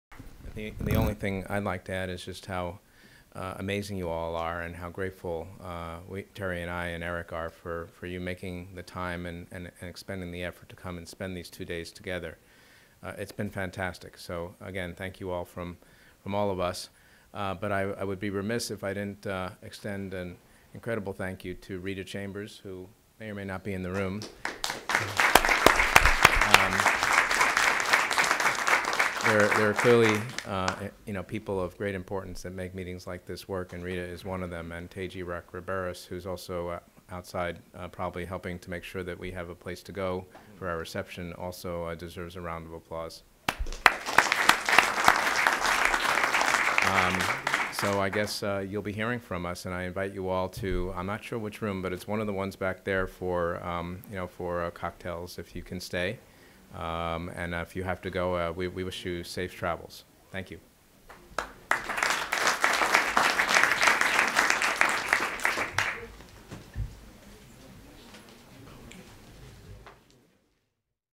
Closing Remarks - Geoffrey Ginsburg
January 9, 2014 - Genomic Medicine Centers Meeting VI: Global Leaders in Genomic Medicine. More